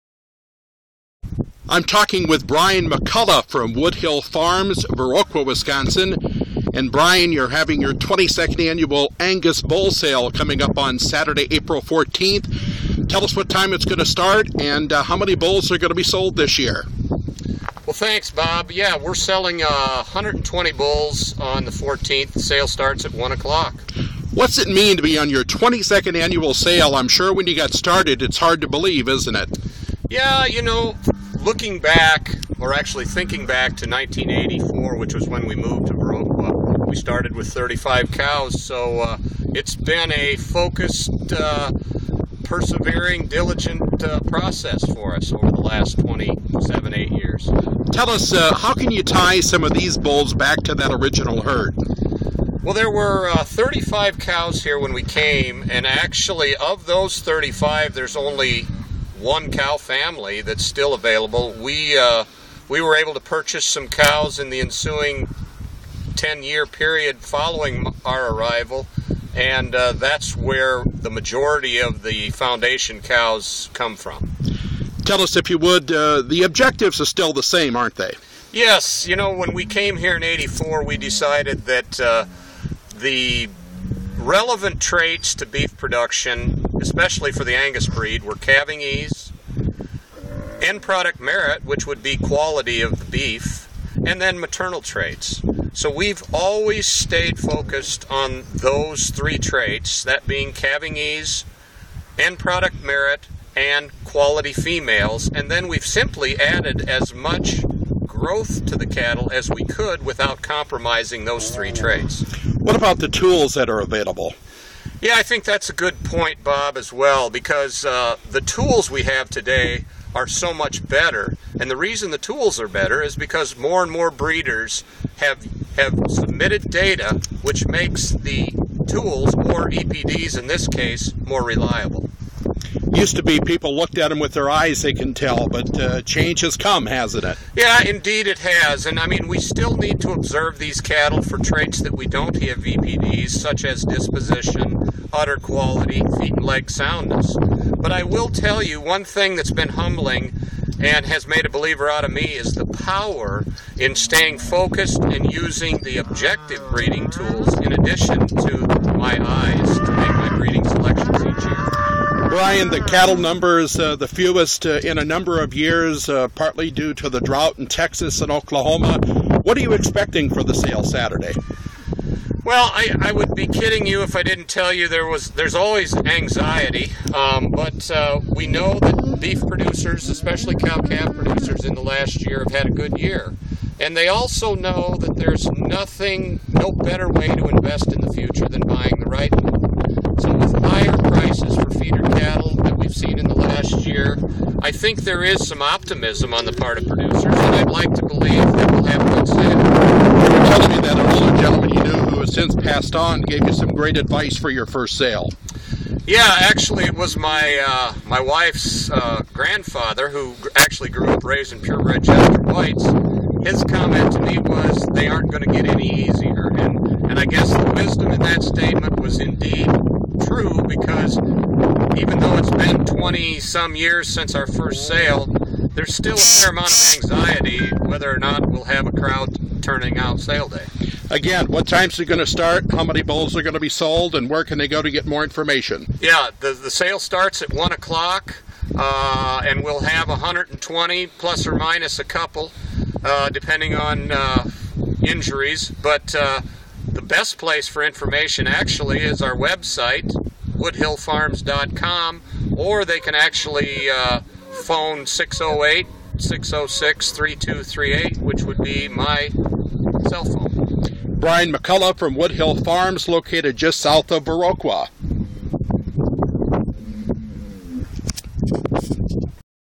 Brian McCulloh-Woodhill Farms
WGLR Radio Farm Director Bob Middendorf interviewing Brian McCulloh of Woodhill Farms-Viroqua about their 22nd Annual Angus Bull Sale-1pm-April 14th at the farm.
Brian also looks back to 1984 when they started with 35 head and their first sale in 1990.